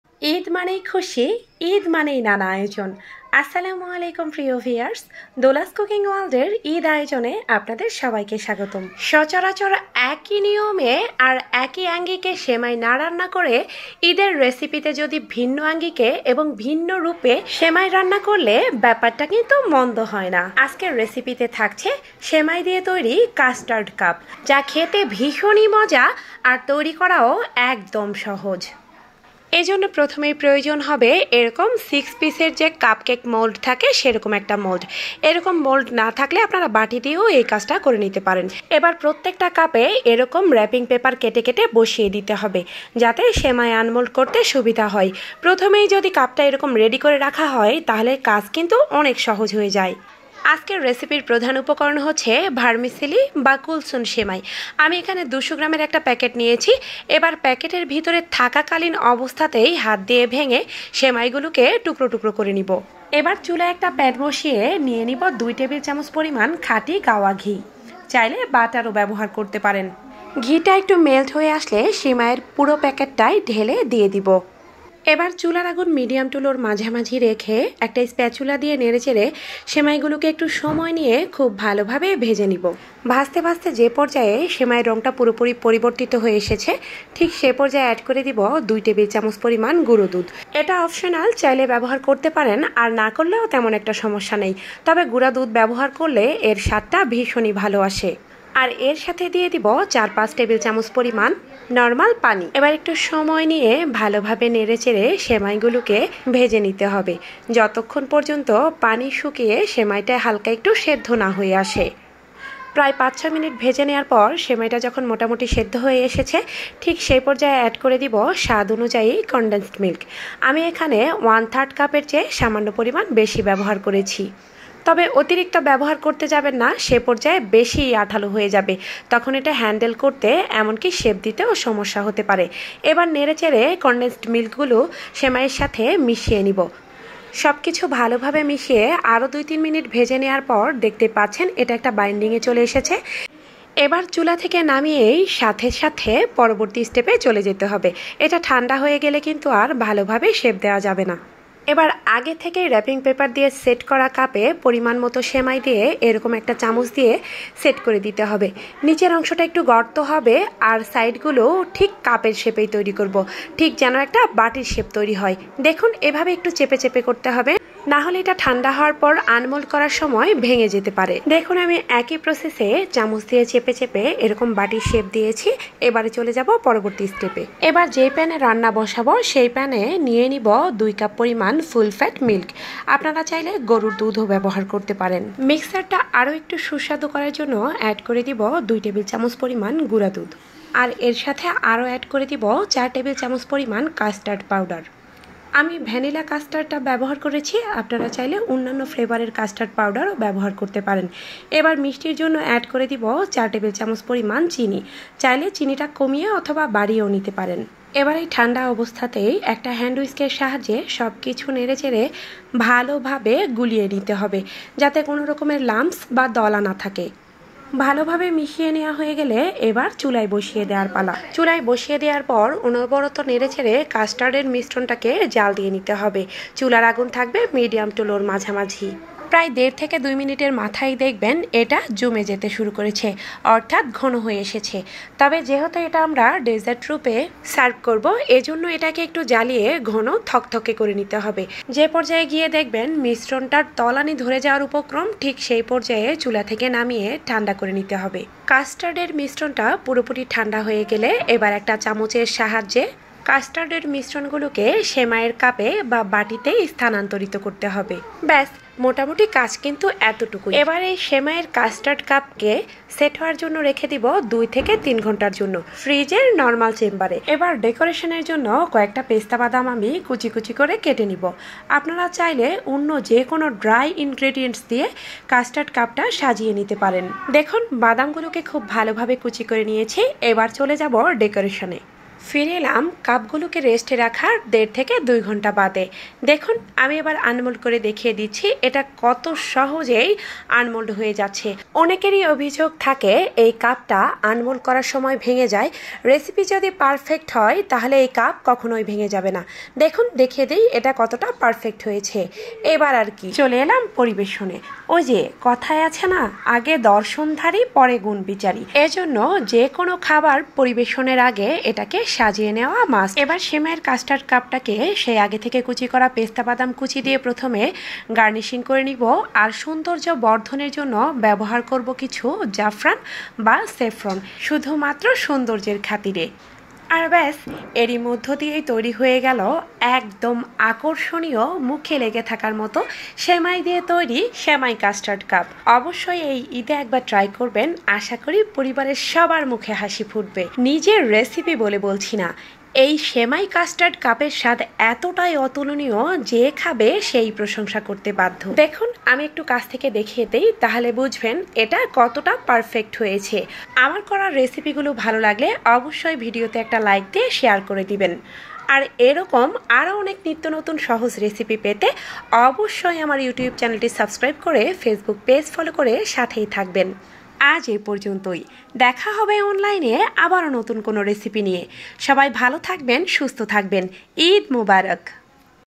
ঈদে সেমাই দিয়ে তৈরি করুন- আকর্ষণীয় ডেজার্ট "সেমাই কাস্টার্ড কাপ" | Vermicelli custard cups
ঈদ স্পেশালে আজকের রেসিপিতে থাকছে, সেমাই দিয়ে তৈরি আকর্ষণীয় ডেজার্ট "সেমাই কাস্টার্ড কাপ, তা তৈরি করা খুবই সহজ,, আর খেতে সত্যিই অসম্ভব মজার,, আশা করি এই ঈদে ট্রাই করবেন,, পরিবারের সবার মুখে হাসি ফুটবে,, সবাইকে ঈদের আন্তরিক শুভেচ্ছা,, ঈদ মোবারক ♥️♥️